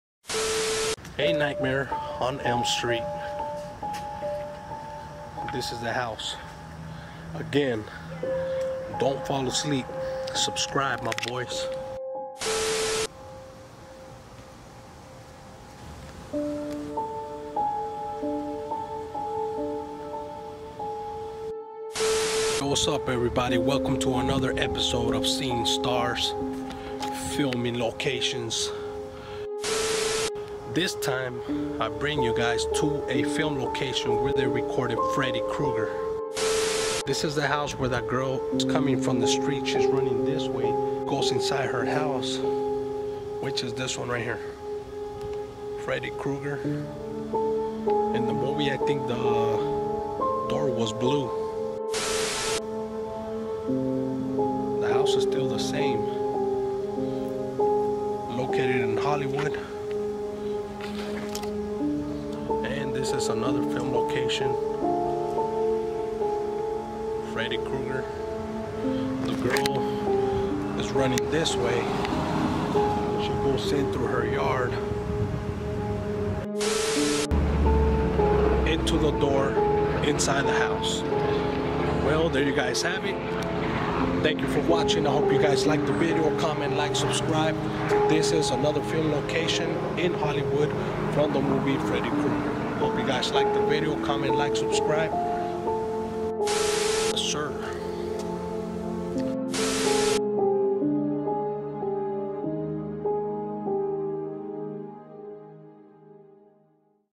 A Nightmare On Elm Street - The Filming Locations
A nightmare on elm street Filming locations
of the filming loctions of nightmare on elm street
this is one of the houses that appears on the movie a nightmare on elm street,